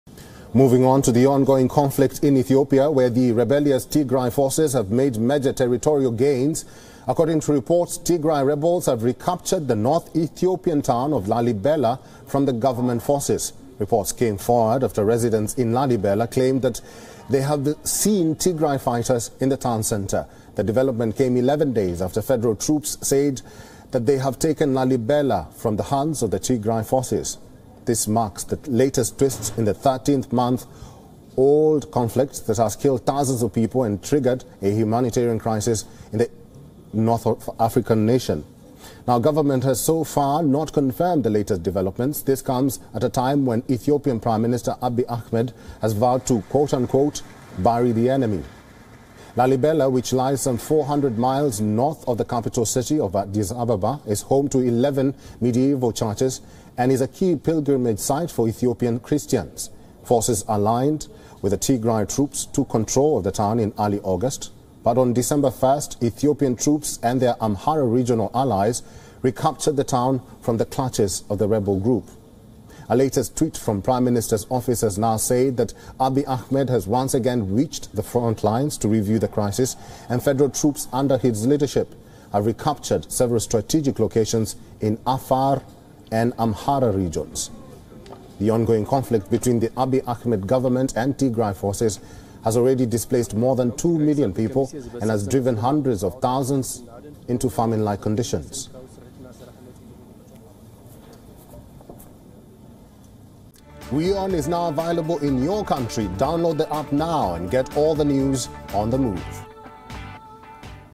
Tigray rebels regain control of Ethiopian heritage town Lalibela | Latest World English News | WION
Tigray rebels on Sunday recaptured the north Ethiopian town of Lalibela, home to a UNESCO world heritage site.